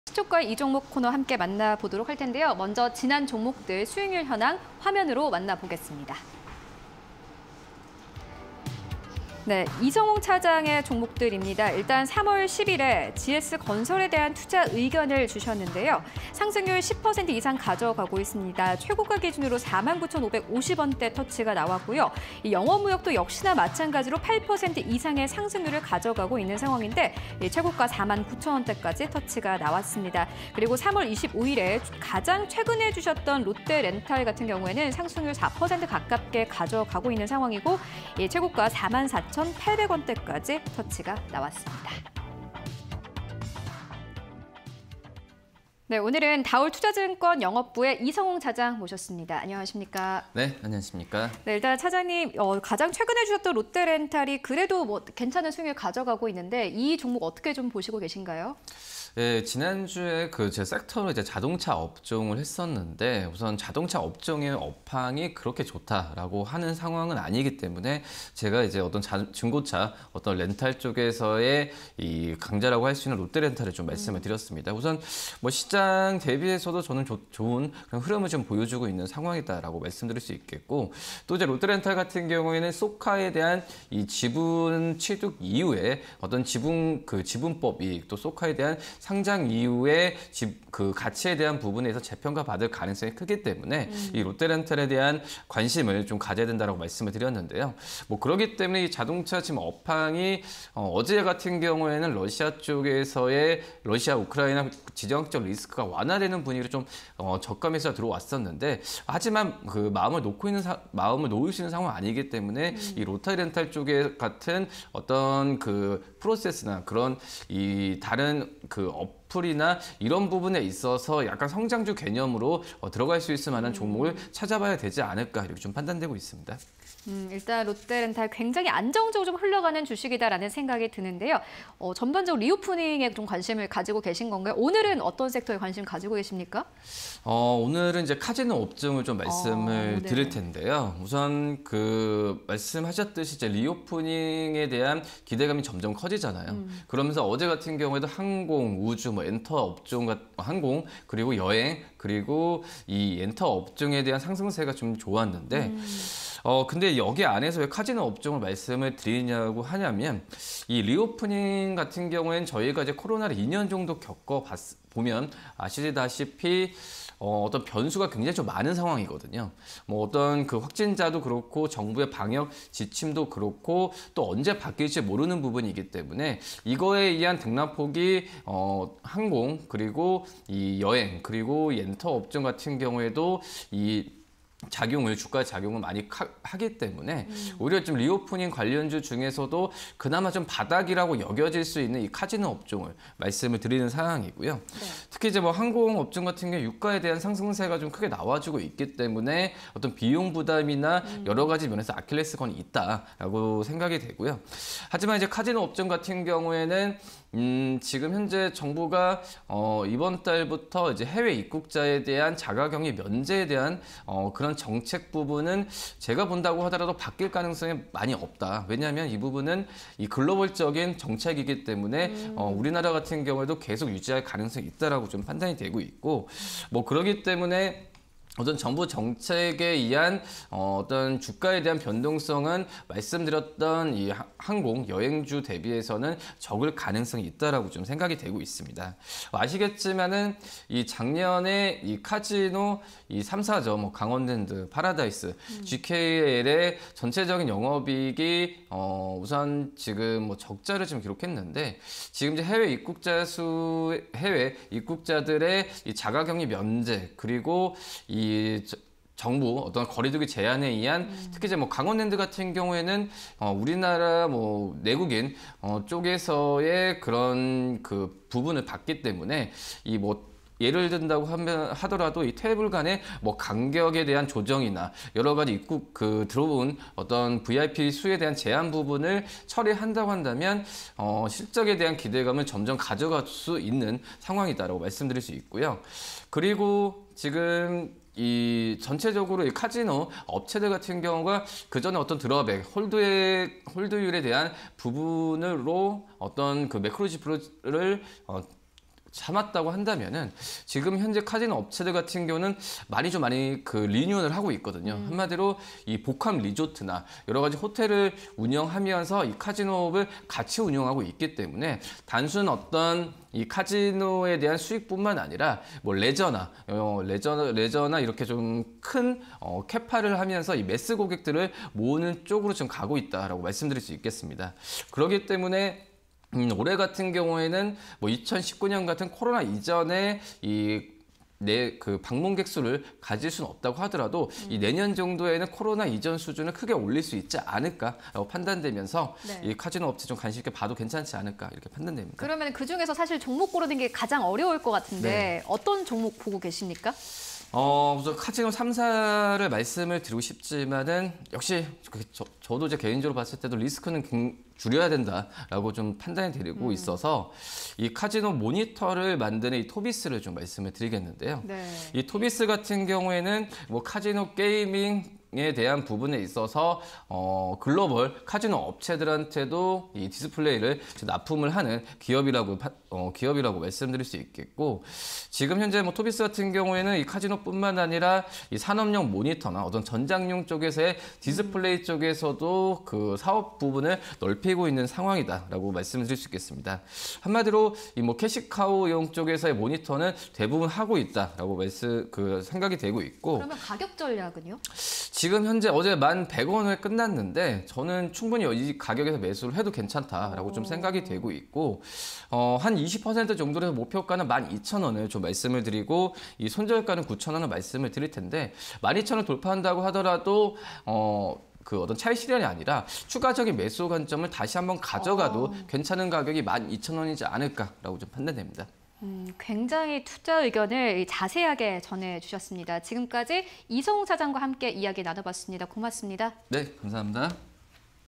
토비스(051360)... 카지노 섹터_시초가에 이 종목 (20220331)
카지노 섹터
- 카지노 방문객, 코로나 이전 수준 회복 예상
- 복합리조트 등 카지노 업체 리뉴얼 긍정적
- 올해 카지노 기업 흑자전환 기대
- 리오프닝 기대감 확대

토비스(051360)
- 디스플레이 올라운드 플레이어
- 카지노·산업용 모니터 제조
- 올해 산업용 모니터 매출 1,669억 원 예상
- 전장 사업 부문 고성장 긍정적